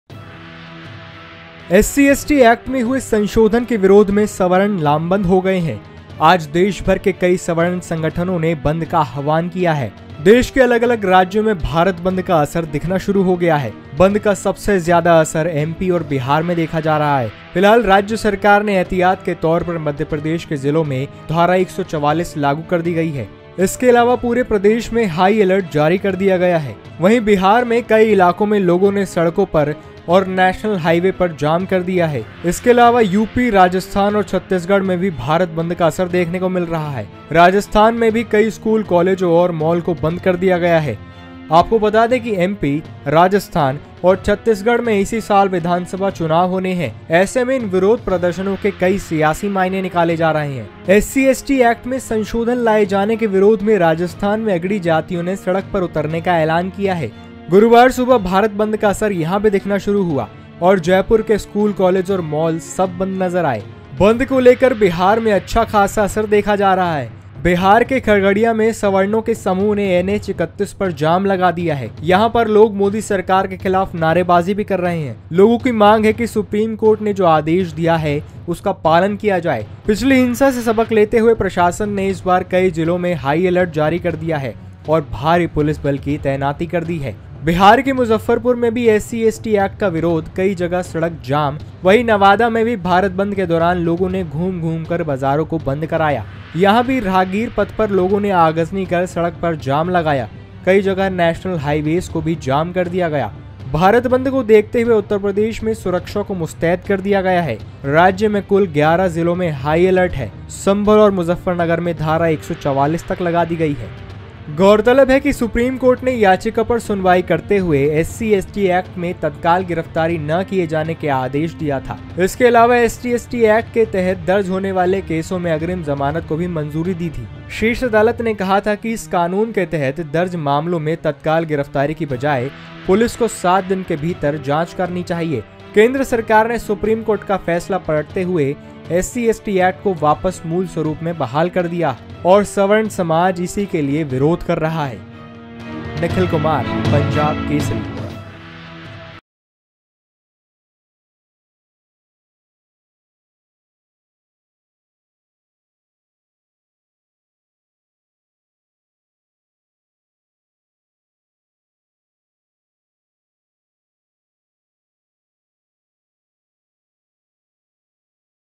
SC/ST ACT के विरोध में सवर्णों का आज देशभर में BHARAT BAND | PunjabKesari.Com
SC/ST ACT के विरोध में सवर्णों का आज देशभर में BHARAT BAND